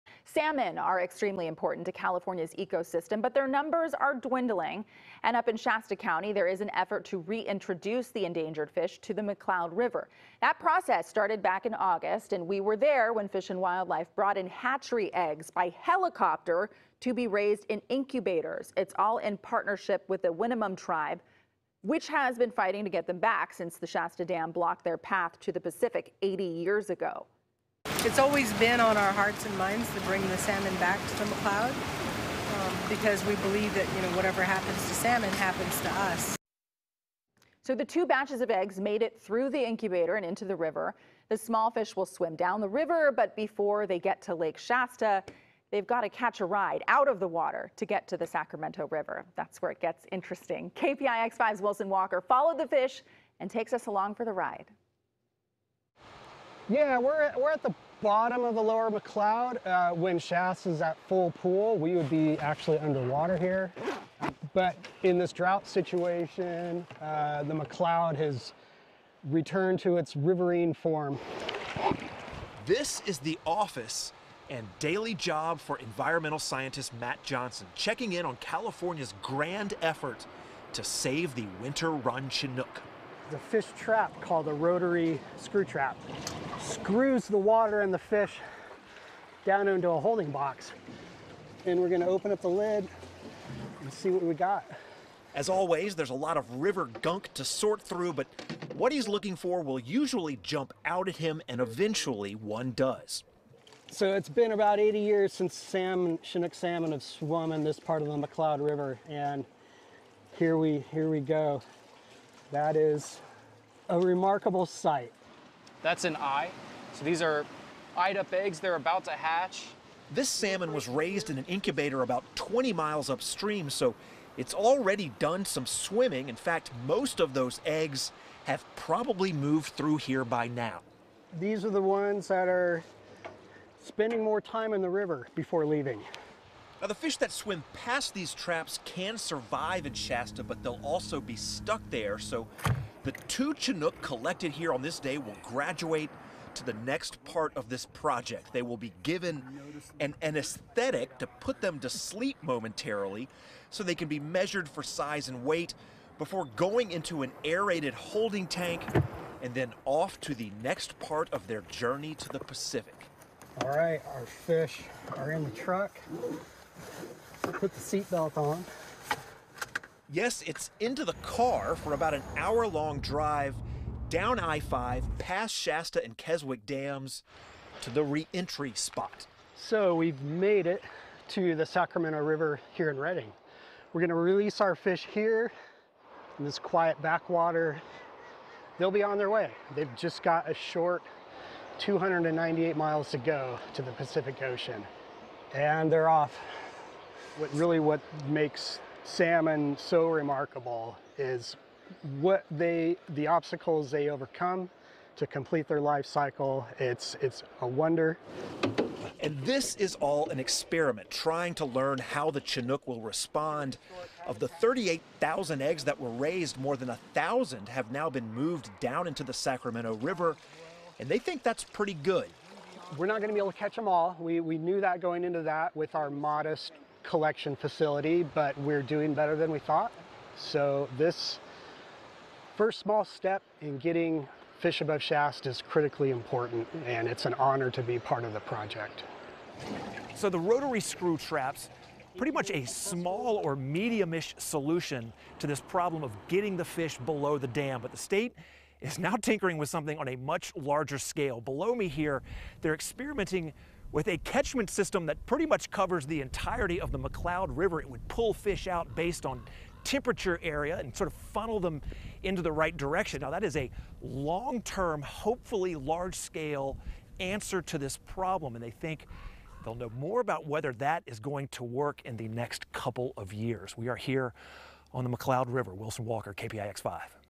Chinook salmon swimming tribe's ancestral McCloud River for first time in decades
A Shasta County tribe has teamed up with federal wildlife workers to reintroduce Chinook salmon to their ancestral river. Wilson Walker got to see the the river's Chinook fry in decades.